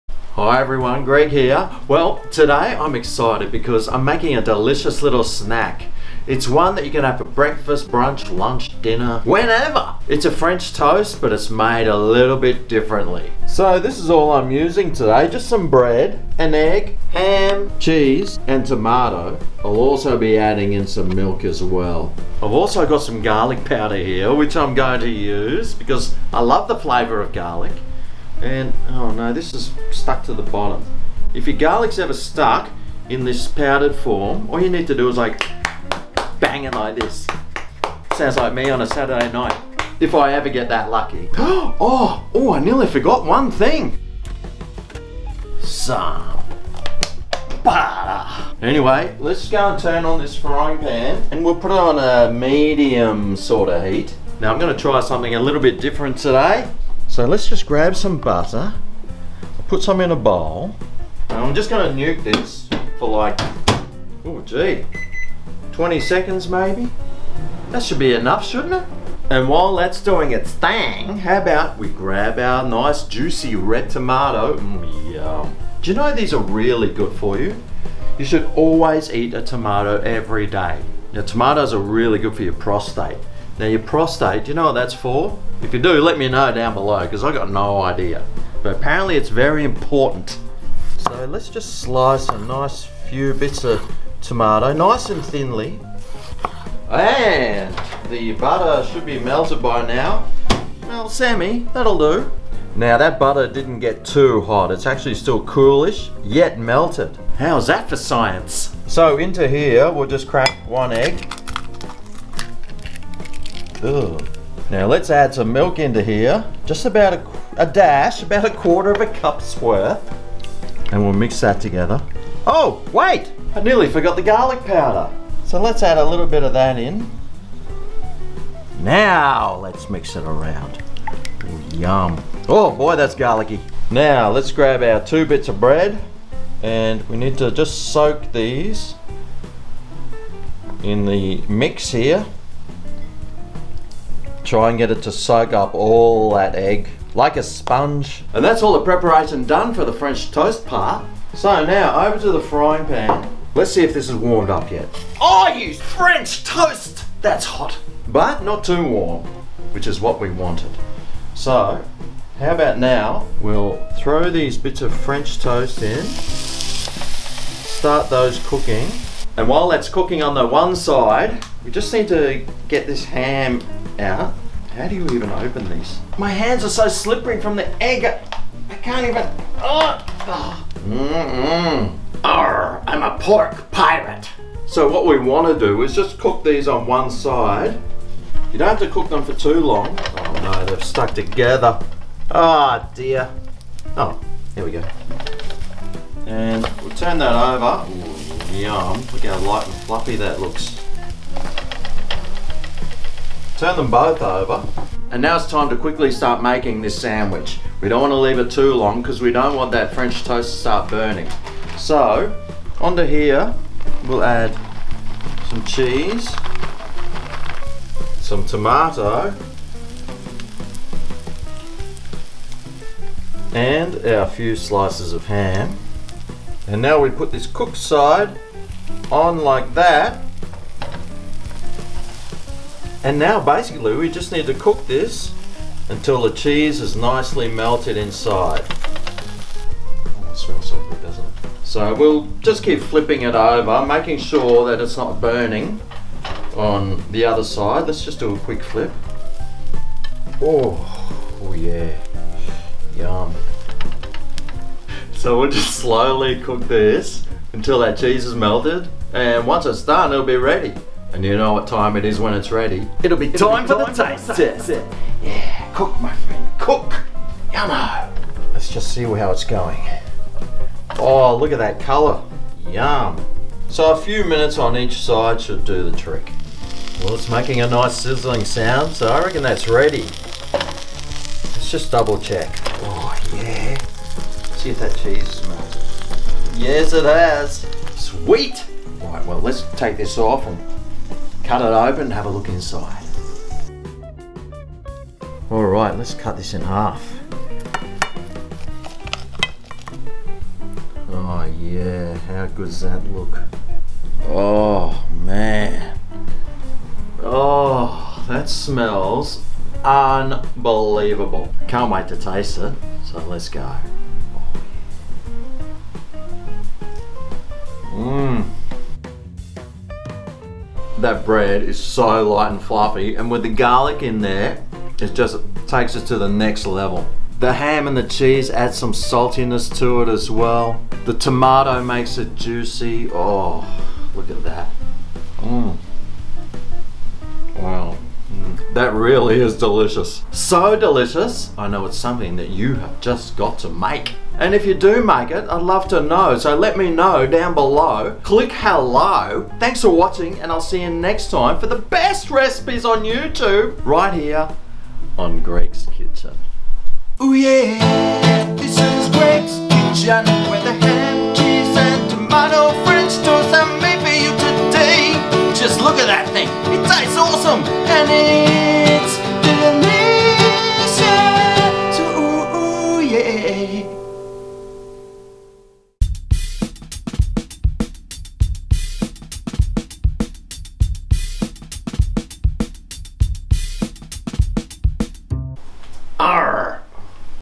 FRENCH TOAST TOASTED BREAKFAST SANDWICH - Greg's Kitchen
Today on Greg's Kitchen l show you how l like to make my favourite ham, cheese and tomato sandwich using a French toast method, the bread comes out light and fluffy and gives this sandwich a real restaurant flavor, l hope you like it!